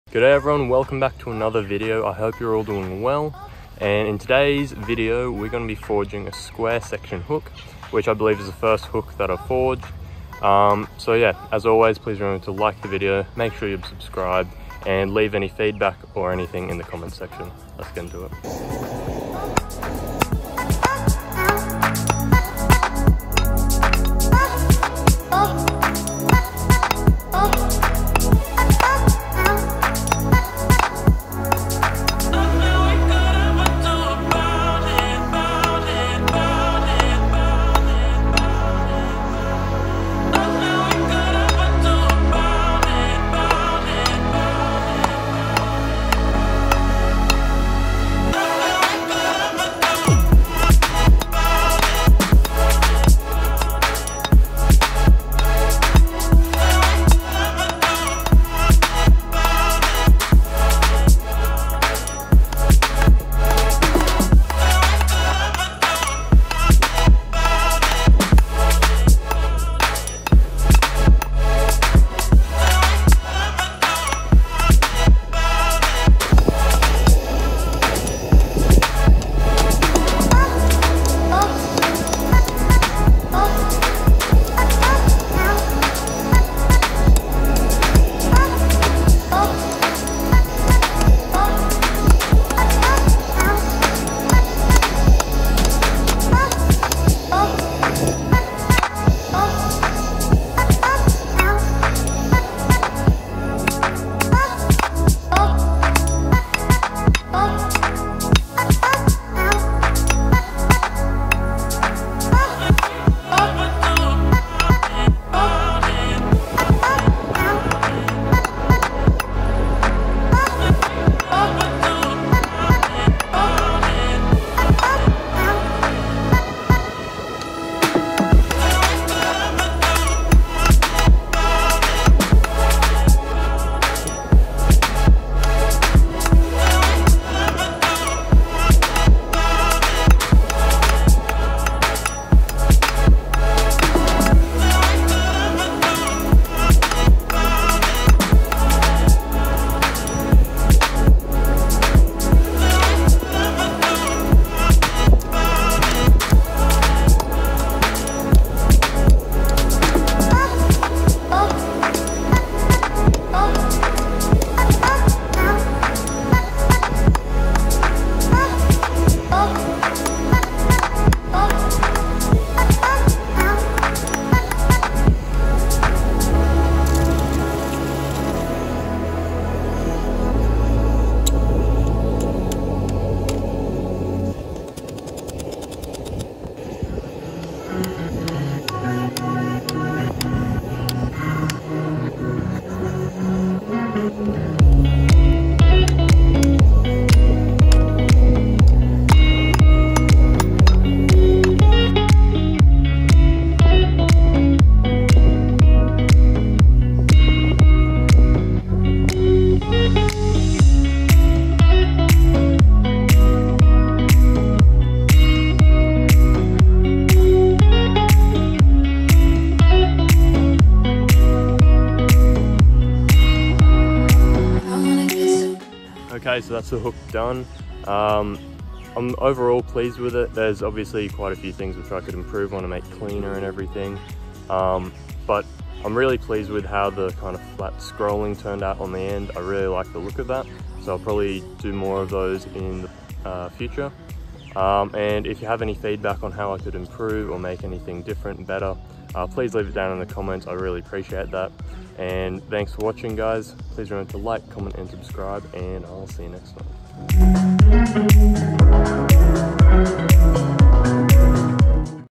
Forging a Square Hook
In this video I forge my first ever hook. This is a square hook, in that it is made from square bar, with a square cross section. Please enjoy!